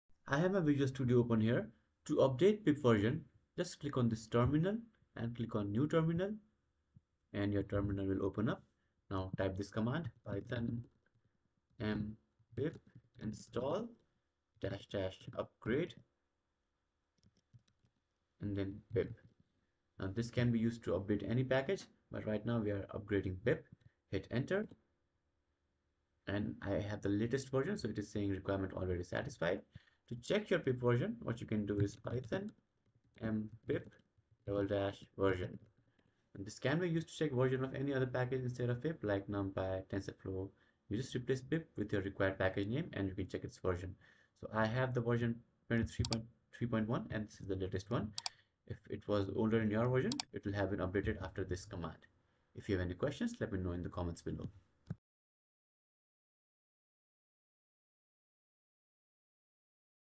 How to Upgrade PIP in Visual Studio Code (2023)
In this video, I'll show you how to Upgrade PIP version in Visual Studio code. PIP version can be update in visual studio code with just one command:
python -m pip install --upgrade pip

Video Topics:
- Upgrade PIP in vscode
- Upgrade PIP command
- How to Upgrade PIP in Visual Studio Code